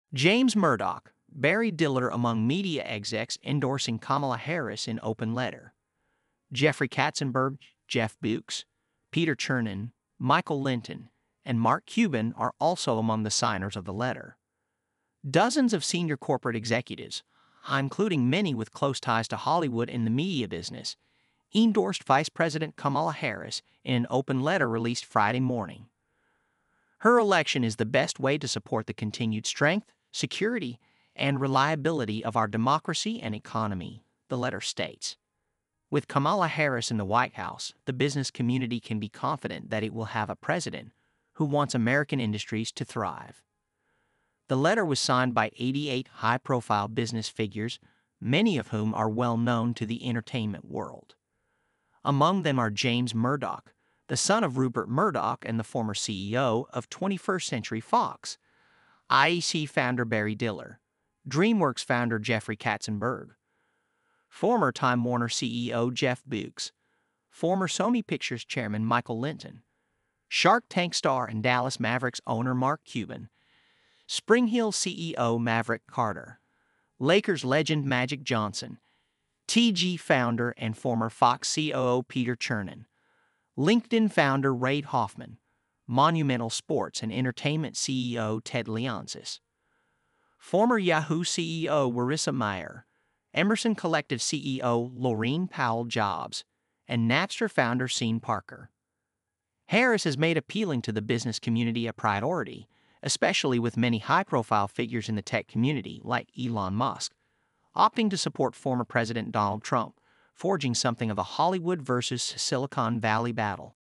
James Murdoch, Barry Diller Among Media Execs Endorsing Kamala Harris in Open Letter
Jeffrey Katzenberg, Jeff Bewkes, Peter Chernin, Michael Lynton, and Mark Cuban are also among the signers of the letter.

Dozens of senior corporate executives—iincluding many with close ties to Hollywood and the media business—eendorsed vice president Kamala Harris in an open letter released Friday morning.